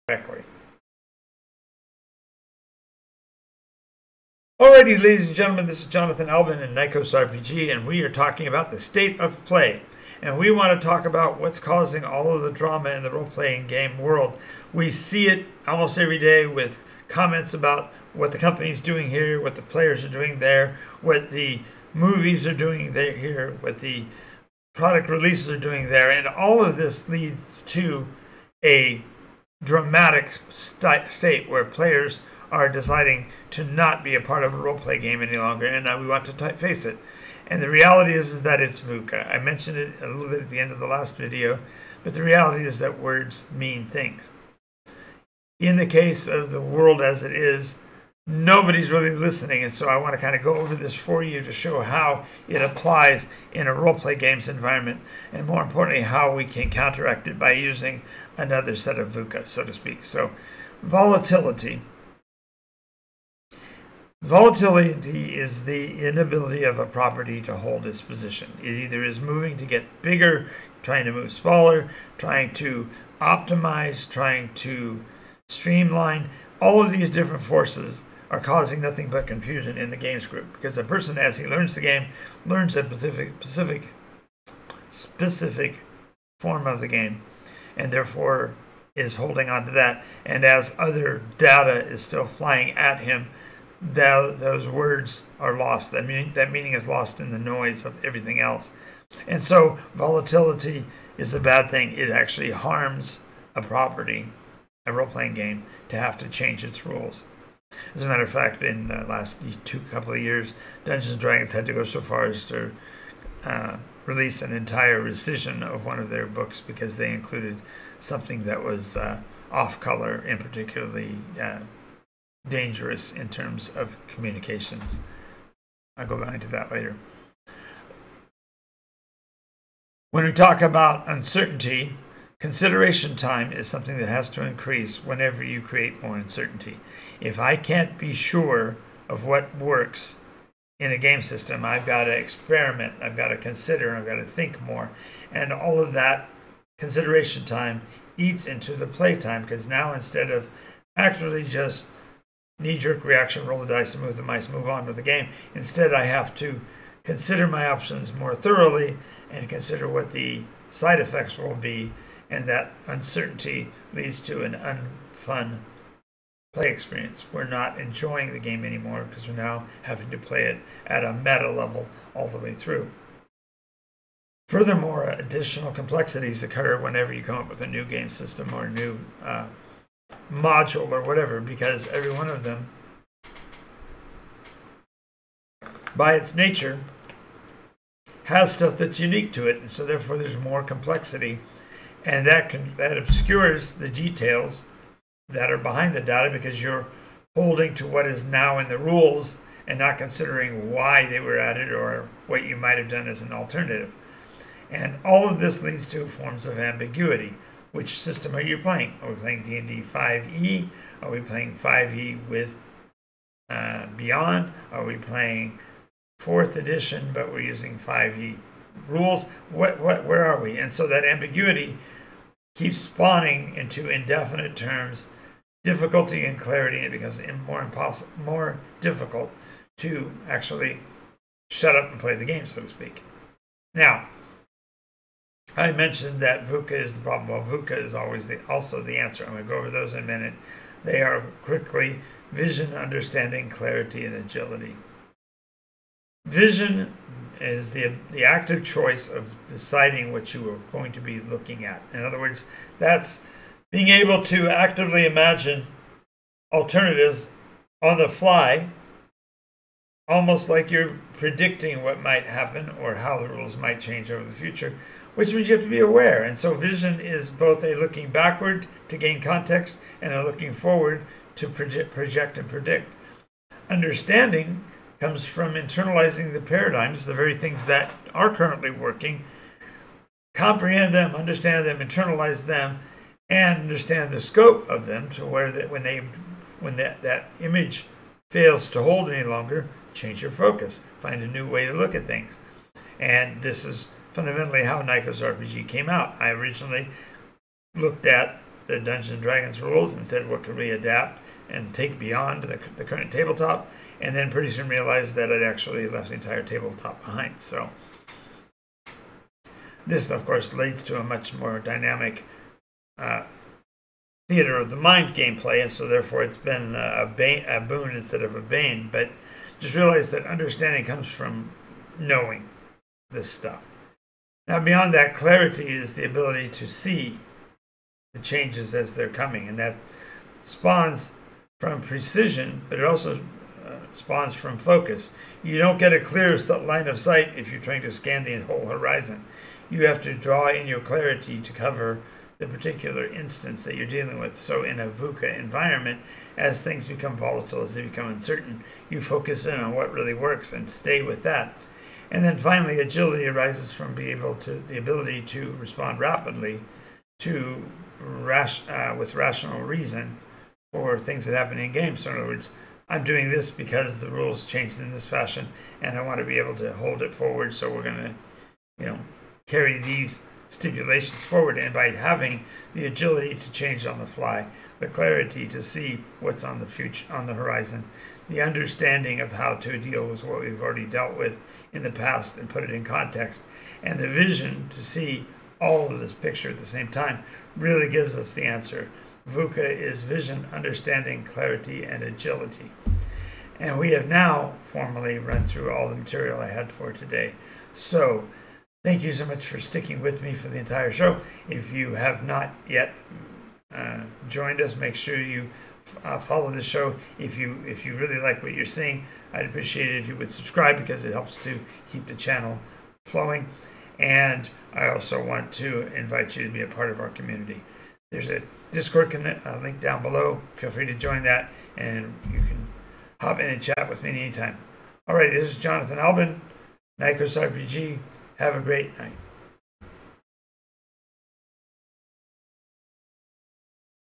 State of Play: What is with all the drama?
Regardless of whether you play RPGS or just listen to the news, you know things are messed up, but maybe, you don&#39;t know why. Game sessions are more strident, and even our workplaces feel more edgy. Here&#39;s the four-letter acronym to explain how, and perhaps give us a way out... -- Watch live